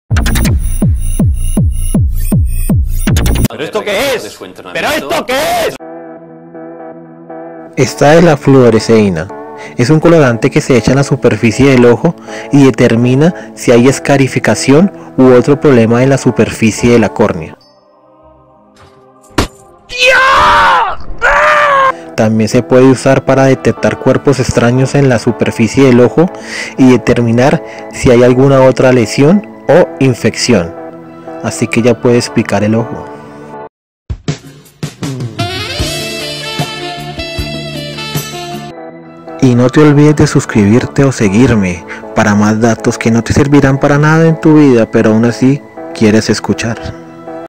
😱 ¿Sabias Que? La Fluoresceína Ayuda a Detectar Problemas en Tus Ojos
El examen de fluoresceína es útil para determinar si hay escarificación u otro problema en la superficie de la córnea. También se puede usar para detectar cuerpos extraños en la superficie del ojo y determinar si hay lesión o infección. El examen se realiza administrando un colorante a la superficie de ojo.